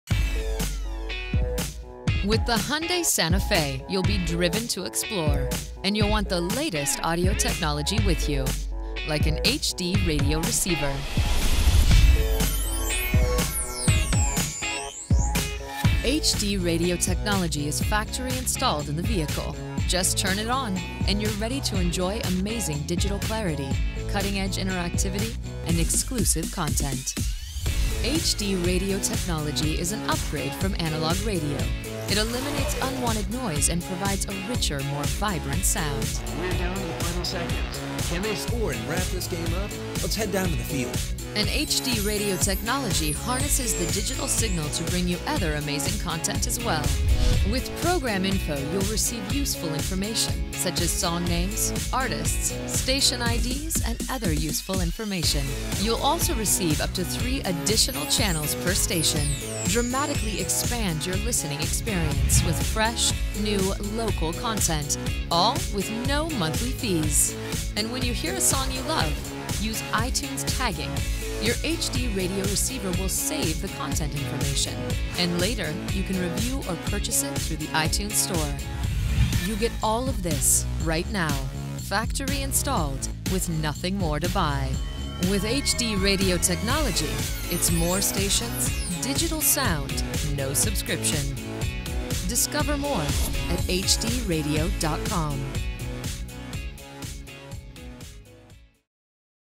2014 & 2015 Hyundai Santa Fe w/HD Radio™ Technology
This video is a tour of the HD Radio receiver that is available in the 2014 & 2015 Hyundai Santa Fe.  HD Radio technology is available as an optional feature in the 2014 & 2015 Santa Fe.